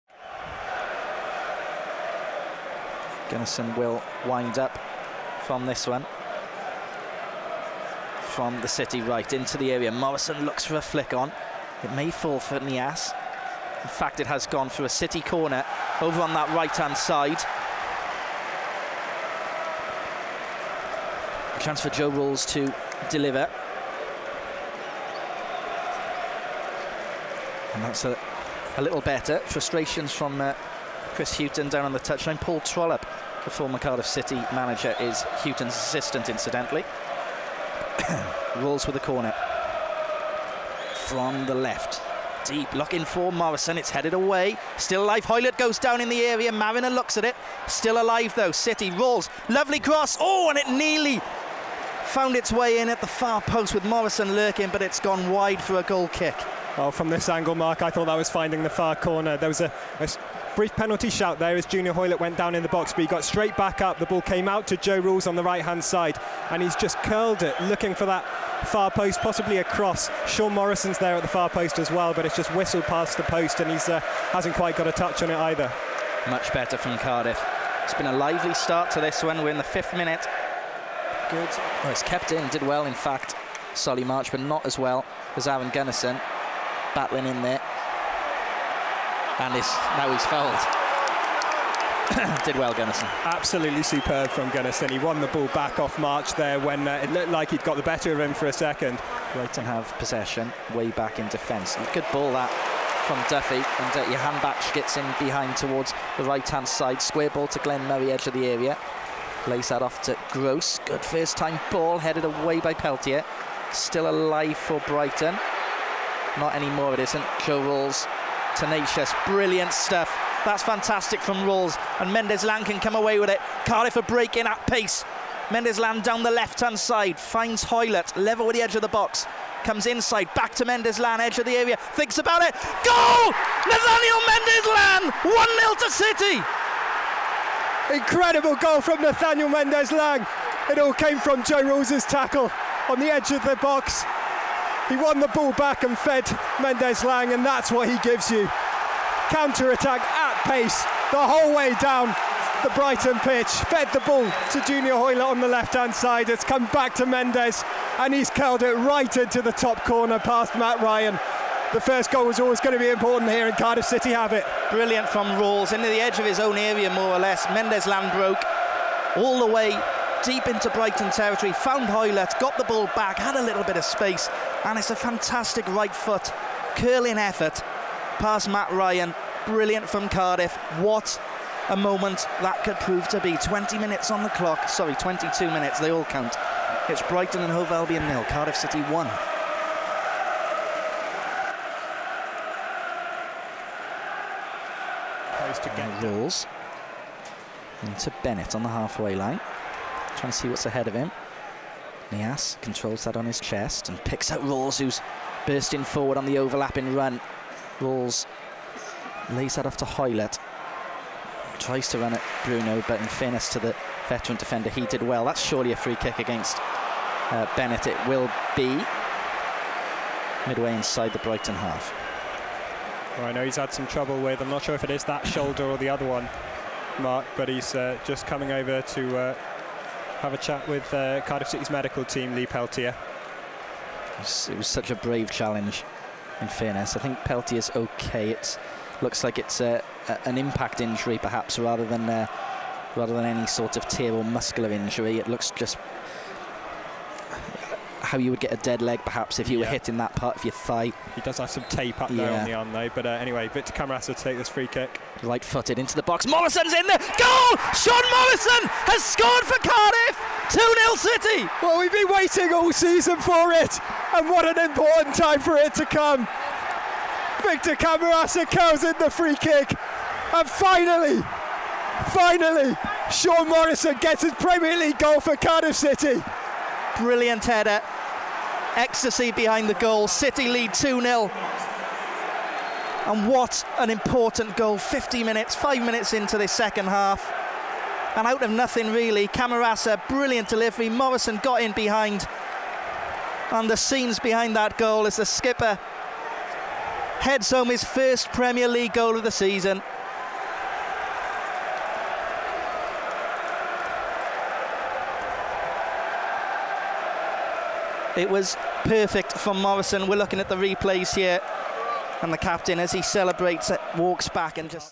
FLASHBACK | BRIGHTON v CARDIFF CITY (2019)
Goals from Mendez-Laing and Morrison give the Bluebirds a chance to stay in the EPL.